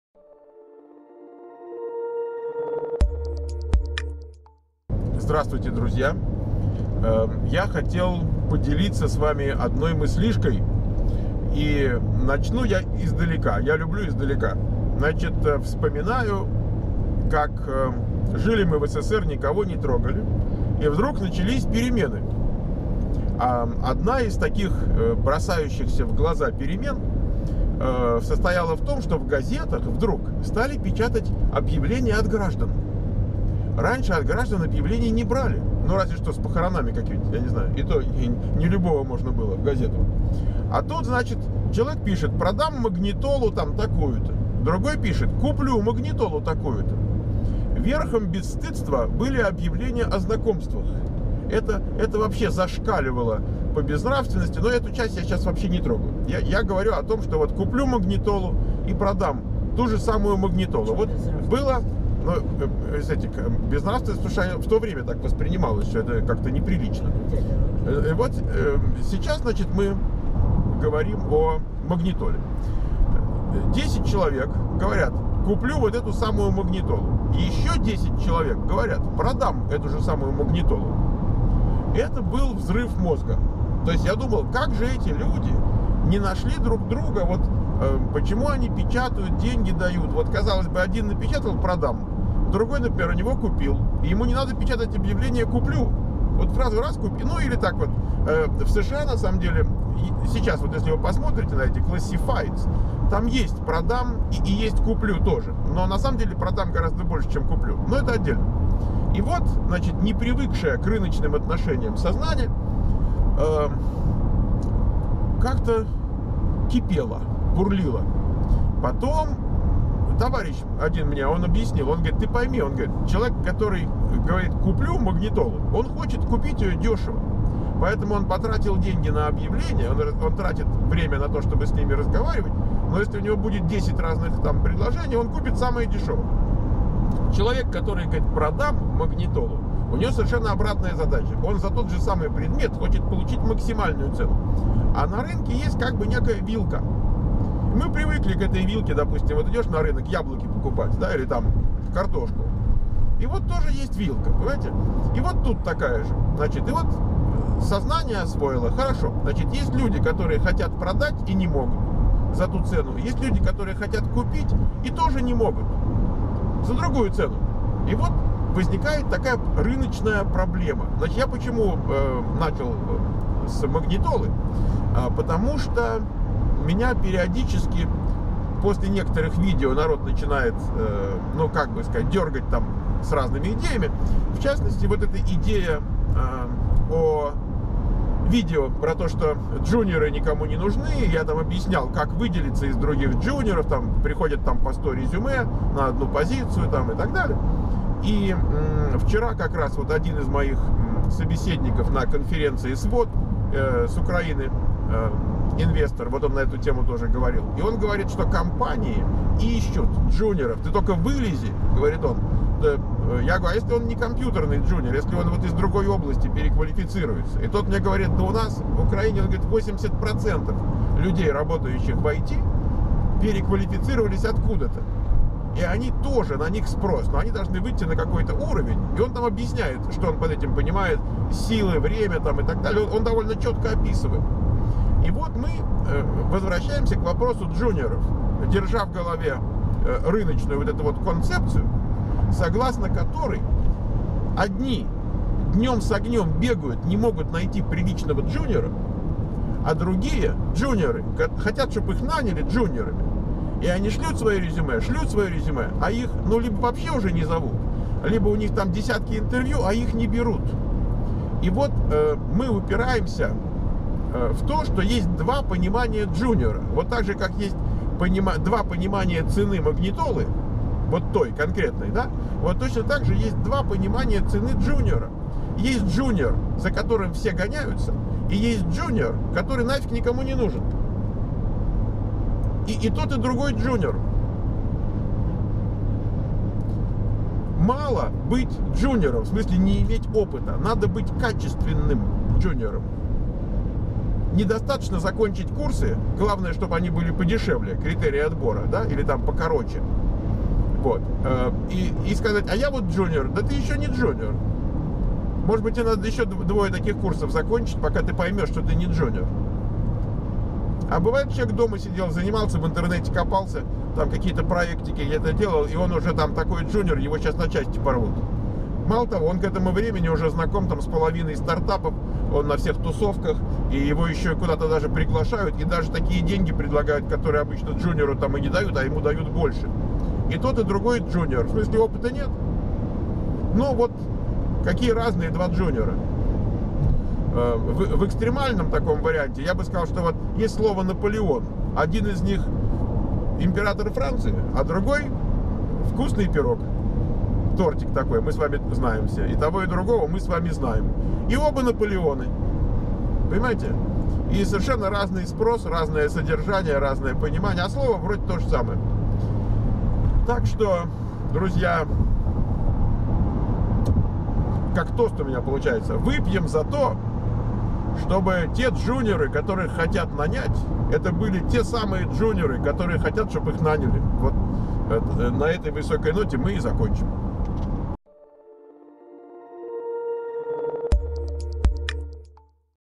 США 3912: И снова о джуниорах - кому и какие реально нужны
И снова о джуниорах - кому и какие реально нужны
Ссылки на канале SiliconValleyVoice /школа Михаил Портнов: